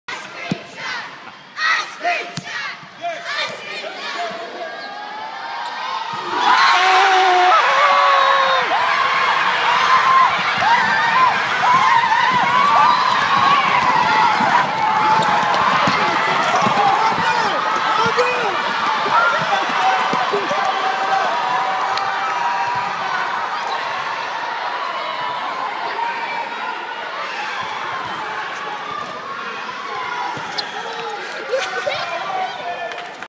Ice Cream Shot - Steve Prohm Basketball Camp
June 17, 2013. Marcus Smith makes 75-foot Ice Cream Shot.